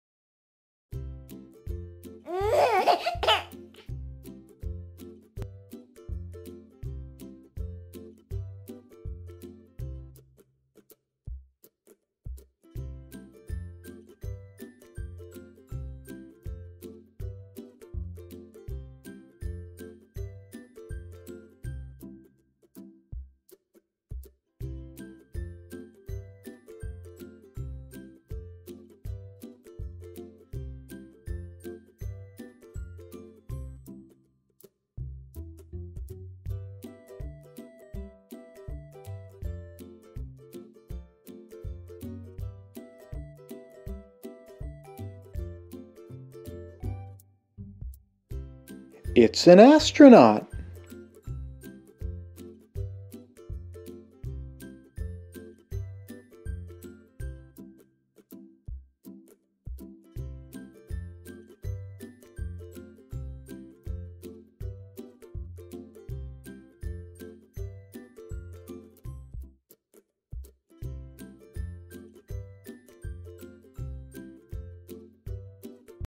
Baby Dragon Themed Puzzle Educational Video for Kids 2
If your child loves fantasy baby dragons, and you love for them to learn, you've found the right educational cartoon video! Learn the times tables (multiplication tables) including for the numbers 1, 2, 3, 4, 5, 6, 7, 8, 9, and 10 with this series of cartoon character parades educational movies for kids!